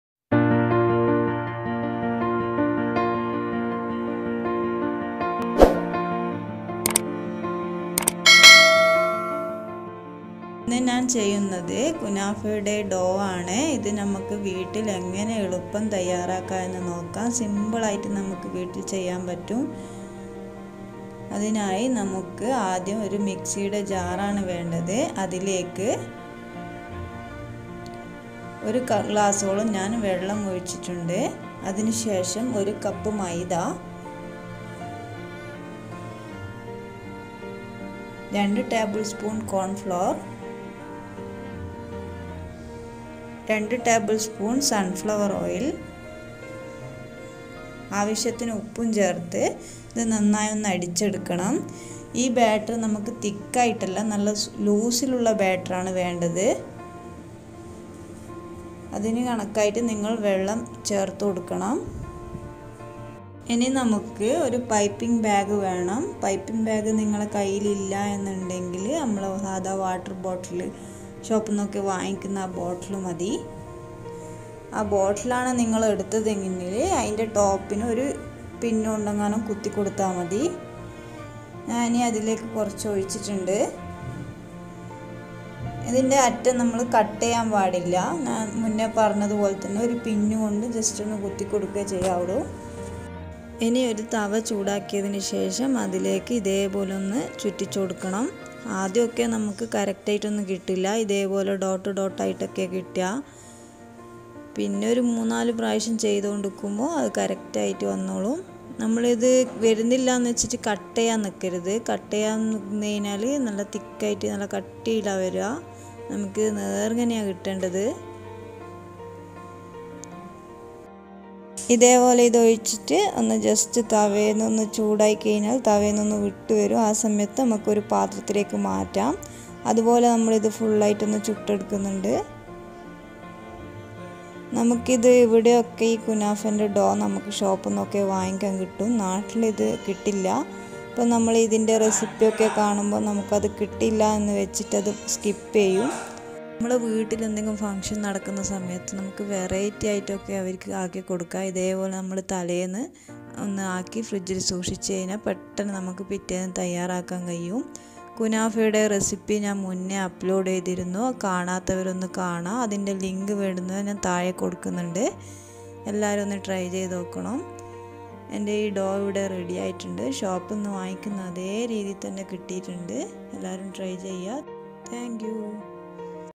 Easy kunafa dough||Homemade|| നാട്ടിലുള്ളവർക്കും ഇനി ഈസിയായി ഇത് വീട്ടിൽതന്നെ ഉണ്ടാക്കിയെടുക്കാം..
Ingrediants
      maida     -1cup
      cornflour -2tbsp
      oil           -2tbsp
      salt
      water